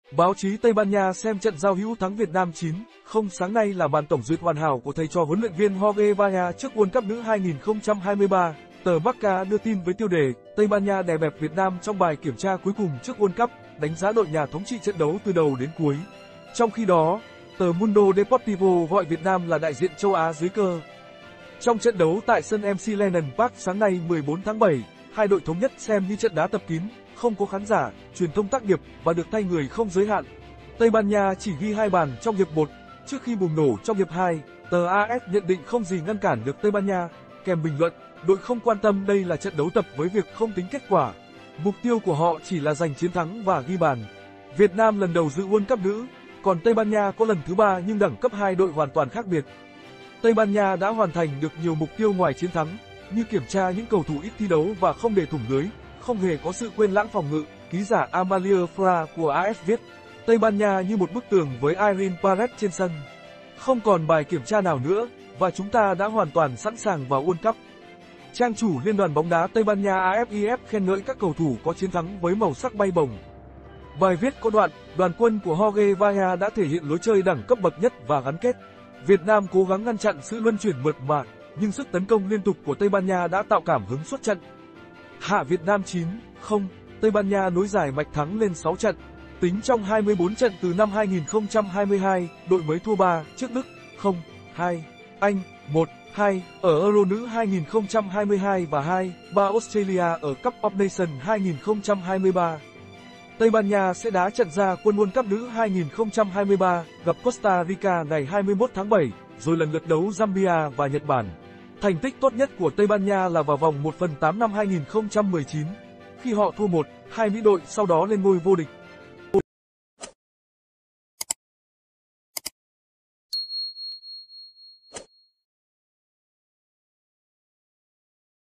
TRUYỀN THÔNG TÂY BAN NHA MÃN NGUYỆN KHI THẮNG ĐẬM VIỆT NAM
Báo chí Tây Ban Nha xem trận giao hữu thắng Việt Nam 9-0 sáng nay là màn tổng duyệt hoàn hảo của thầy trò huấn luyện viên Jorge Vilda trước World Cup nữ 2023.Tờ Marca đưa tin với tiêu đề "Tây Ban Nha đè bẹp Việt Nam trong bài kiểm tra cuối cùng trước World Cup", đánh giá đội nhà thống trị trận đấu từ đầu đến cuối. Trong khi đó, tờ Mundo Deportivo gọi Việt Nam là đại diện châu Á dưới cơ.